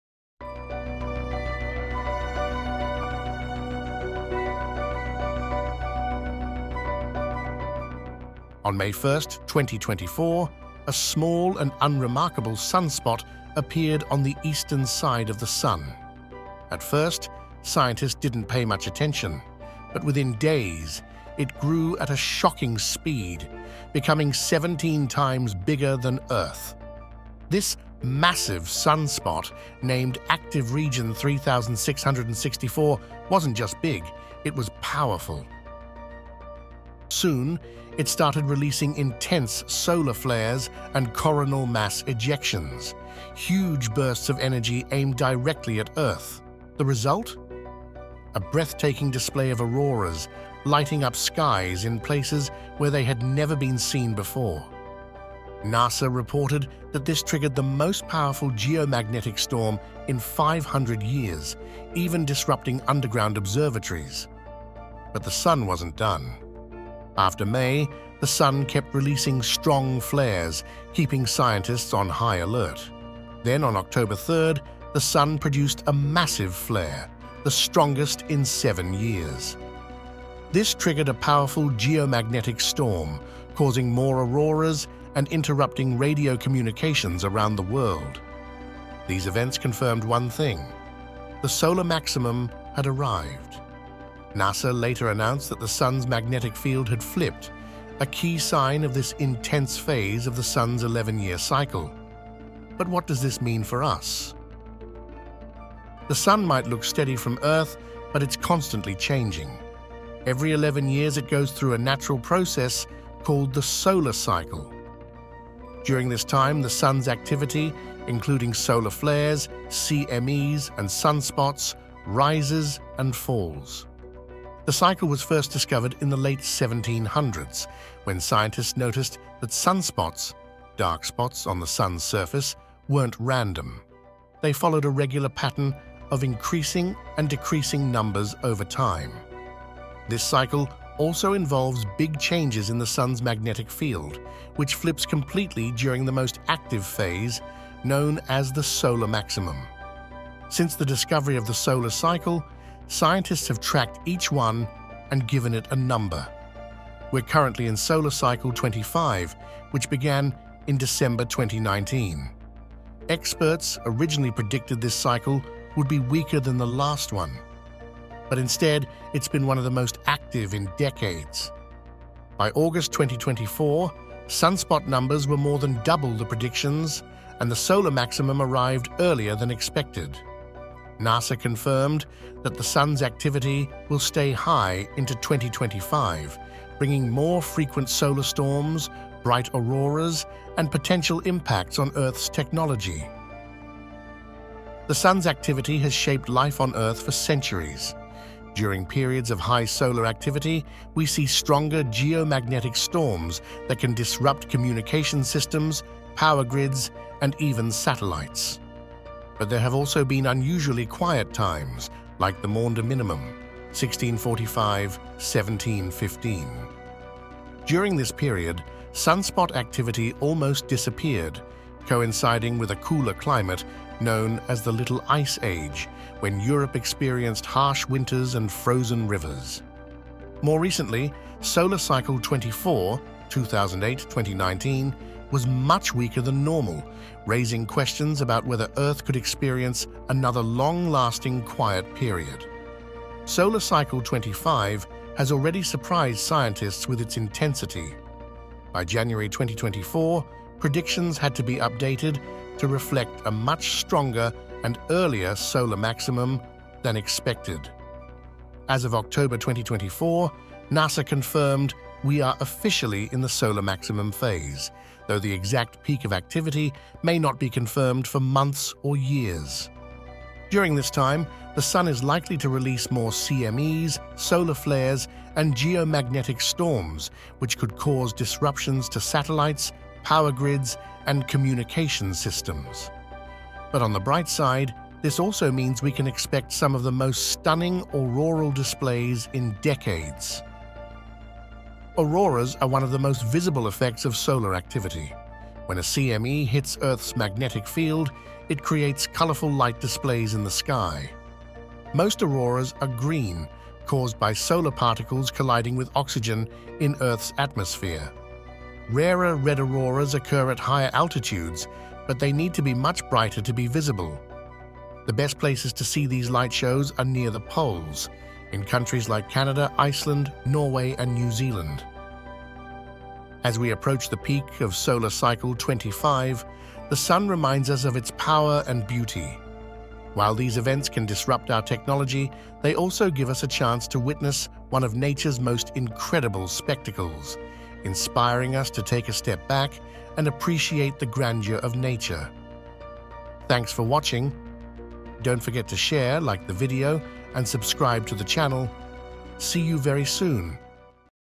"Earth on Alert: The Sun’s Magnetic Flip Marks the Start of Solar Chaos!"
NASA has officially confirmed that the Sun has reached the peak of its 11-year cycle, marking the start of the solar maximum phase of Solar Cycle 25. But what does this heightened solar activity mean for Earth in the months ahead? How long will this intense period of solar flares and geomagnetic storms last? And perhaps most importantly, could this surge in solar energy impact global temperatures and contribute to warming our planet in the year to come?